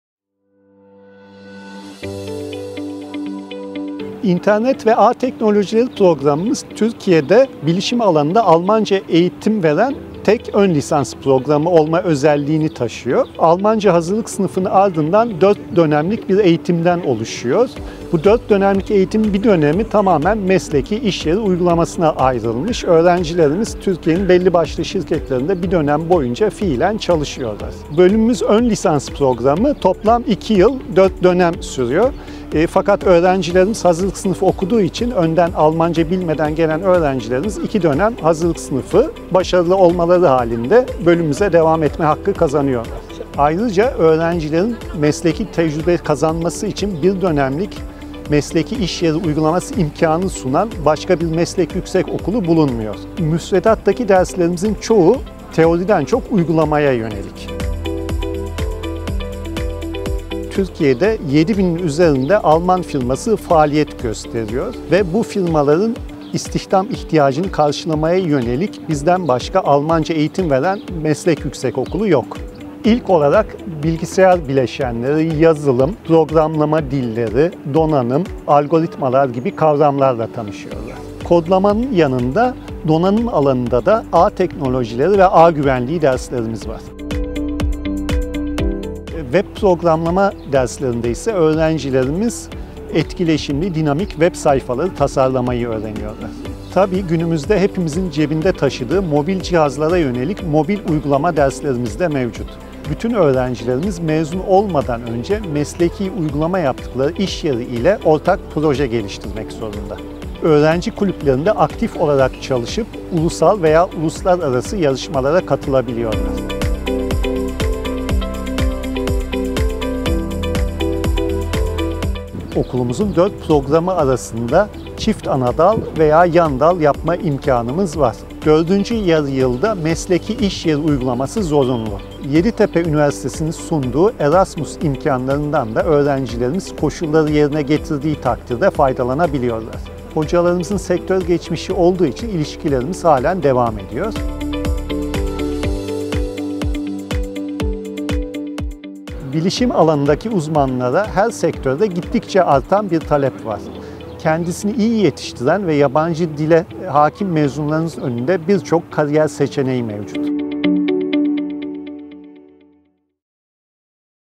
Yeditepe Üniversitesi İnternet ve Ağ Teknolojileri Bölümü
Yeditepe Üniversitesi İnternet ve Ağ Teknolojileri Bölümü hakkında merak edilenleri  İnternet ve Teknolojileri  Bölümü Başkanı Öğr. Gör. Emin Deniz Anık anlatıyor.